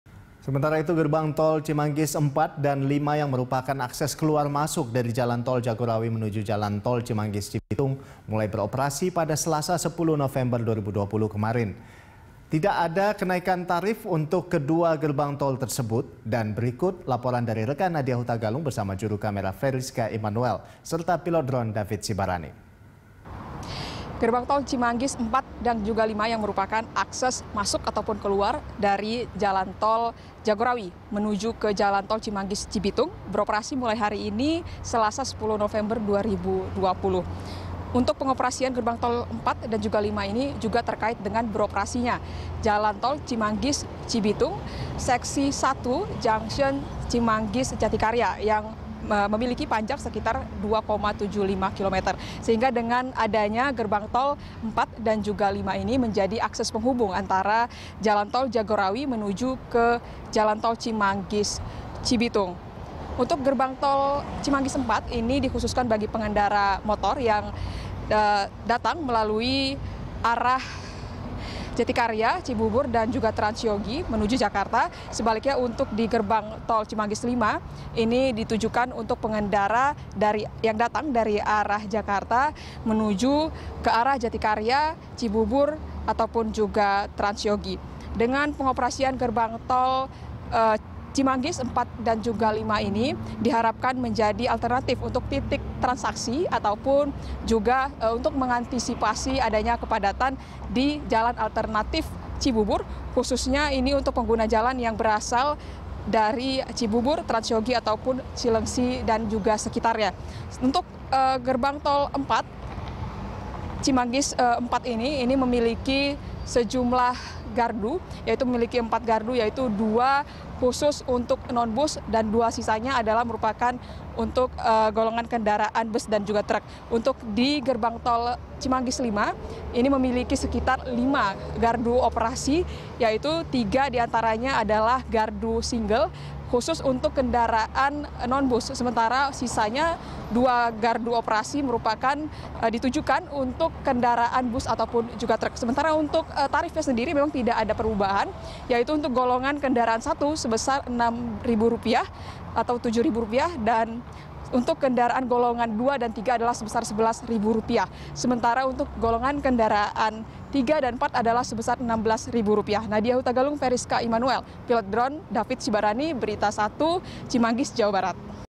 Gerbang Tol Cimanggis 4 dan 5 Beroperasi, Tarif Tetap
Gerbang Tol Cimanggis 4 dan 5 yang merupakan akses keluar masuk dari jalan Tol Jagorawi menuju jalan Tol Cimanggis-Cibitung mulai beroperasi Selasa (10/11) kemarin. Sementara itu, tidak ada kenaikan tarif untuk kedua gerbang tol tersebut.